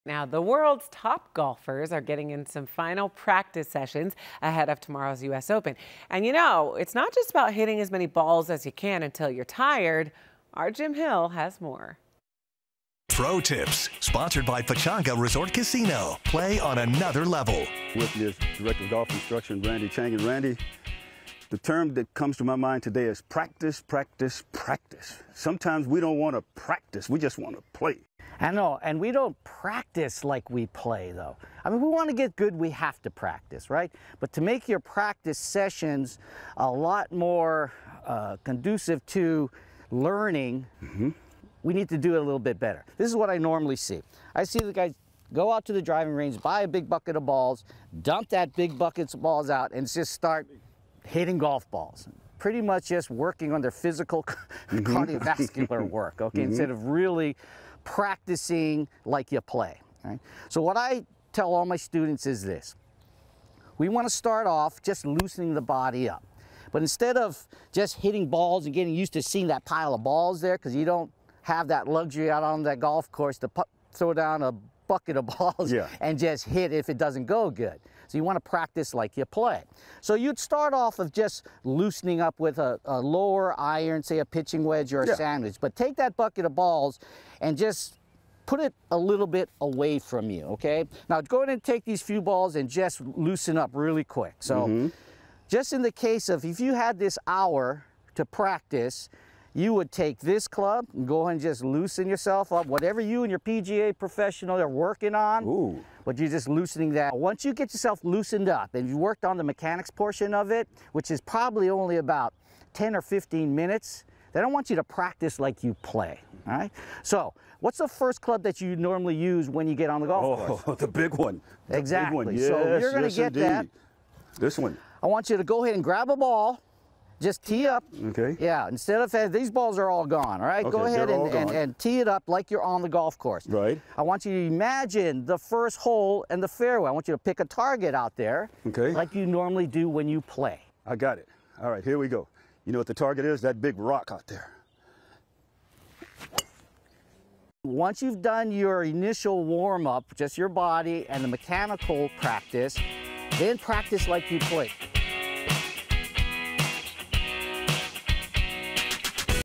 Pro Tips - Practice!
"Practice?!" For golfers, practice may not make perfect, but it sure helps a whole lot. Now, you can make the most out of your time on the range with this Pro Tip, as Randy Chang guides us on how to practice like you play